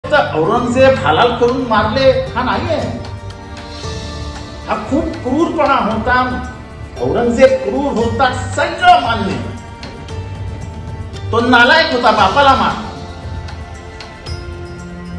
NAUZBILLAH JITENDER AWHAD NE HAZRAT AURANGZEB AALAMGIR ( REHMATULLAHI ALEHA ) KO BATAYA NALAYAK
Nauzbillah... *गुस्ताख NCP MLA जितेंद्र अवहाड ने हज़रत औरंगज़ेब आलमगीर (रहमतुल्लाह अलैहे) को बताया नालायक.* 😡

अपने ज़मिर को ज़िंदा करो MLA जितेंद्र अव्हाड पार्टी के मुसलमान नेता और कार्यकर्ताओं, तुम्हारा So Called Secular नेता अब औलिया इकराम को गाली भी दे रहा है.

इसके पेहले MLA जितेंद्र अवहाड ने हज़रत औरंगज़ेब आलमगीर (रहमतुल्लाह अलैहे) को कहा था ज़ालिम और अपने बाप भाई का हत्यारा. याद रहे 2009 से कलवा-मुंब्रा विधानसभा से मुसलमानों के वोटों से जीत कर आने वाले NCP MLA जितेंद्र आव्हाड लगतार औलिया हज़रत औरंगज़ेब आलमगीर (रहमतुल्लाह अलैहे) की शान में गुस्ताखी कर रहे हैं और मुंब्रा के उलमा ए इकराम, मुफ्ती व पढ़ा लिखा तबका आंखें बंद कर बैठा है.

MLA जितेंद्र अवहाड द्वारा बार बार गुस्ताखी सिर्फ और सिर्फ एक विशेष समुदाय को खुश करने के लिए किया जा रहा है और करोडों मुसलमानों की आस्था की धजियां उढ़ायी गई।

*अफसोस...* हम कौनसे दर्जे के मुसलमान होते जा रहे है ??? औलिया इकराम की गुस्ताखी के बाद भी हमारी आँखें नहीं खुल रही ???

*NCP SP पार्टी मूर्दाबाद*
*MLA ज़ितेन्द्र अवहाड मूर्दाबाद*